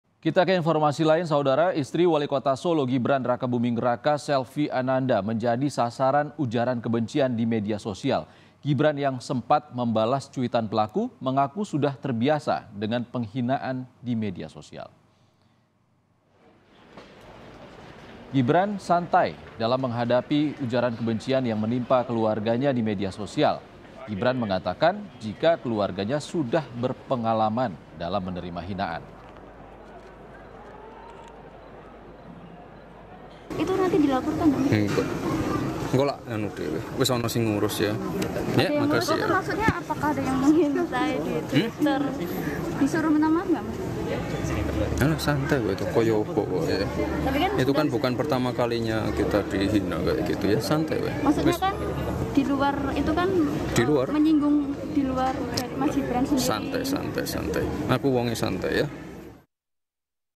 Soal Penghinaan pada Istrinya di Medsos, Gibran: Aku Wongnya Santai
Gibran mengatakan jika keluarganya sudah berpengalaman, dalam menerima hinaan.

''Itu kan bukan pertama kalinya kita dihina kaya gitu, santai'', ujar Gibran.

''Aku wongnya santai yaa'', tambahnya.

Baca Juga Respons Ganjar Pranowo Terkait Pertemuan Gibran dan Puan di Solo di